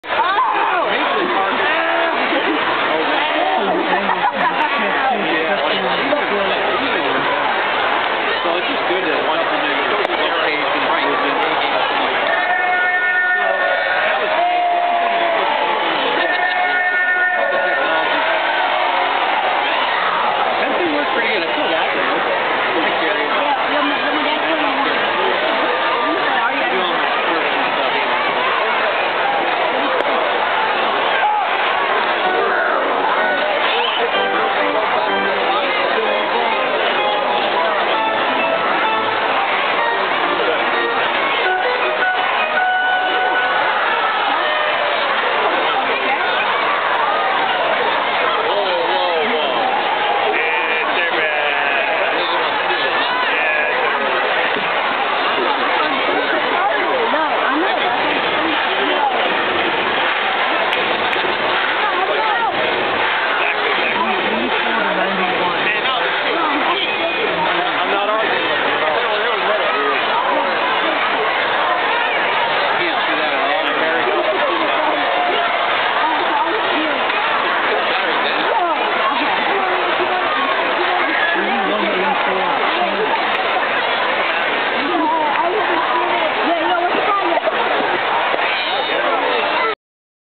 Jared Wesson strikes out Ronnier Mustelier
Erie pitcher Jared Wesson strikes out Thunder third baseman Ronnier Mustelier on April 26, 2012.